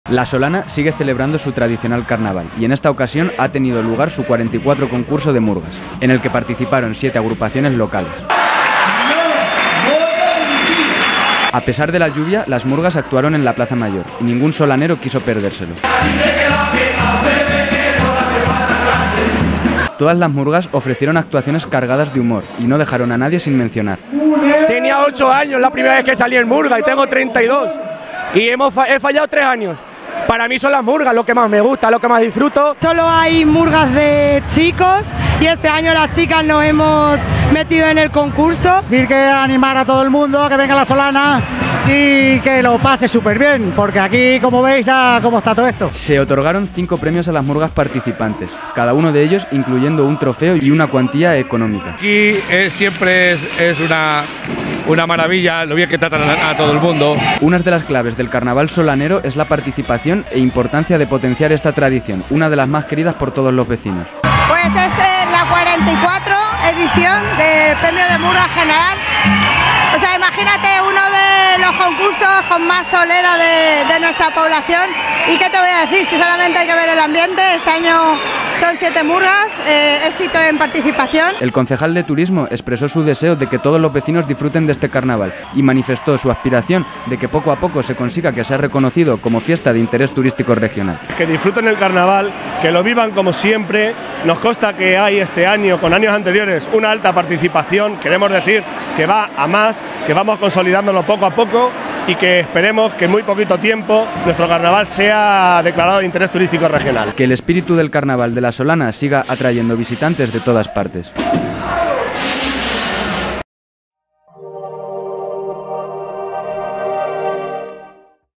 La Solana celebra su concurso de Murgas en medio del carnaval
La Solana sigue celebrando su tradicional carnaval y en esta ocasión ha tenido lugar su 44 concurso de Murgas, en el que participaron 7 agrupaciones locales. A pesar de la lluvia, las murgas actuaron en la Plaza Mayor, y ningún solanero quiso perdérselo. Todas las murgas ofrecieron actuaciones cargadas de humor y no dejaron a nadie sin mencionar.